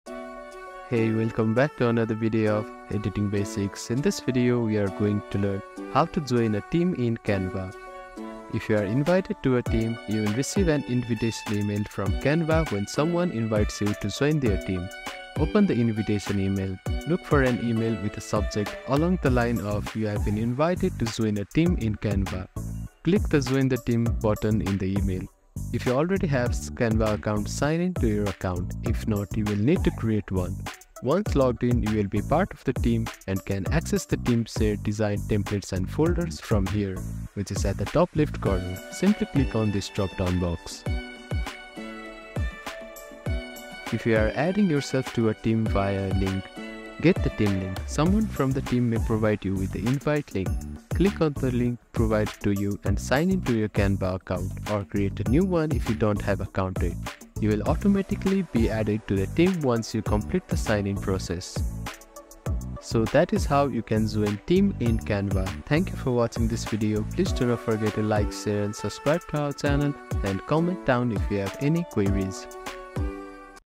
How to Join Team in Canva 2024?
In this video, I’ll show you how to join a team in Canva, allowing you to collaborate effectively on design projects with others. Whether you're working on a project for school, work, or a personal endeavor, this tutorial will guide you through the process!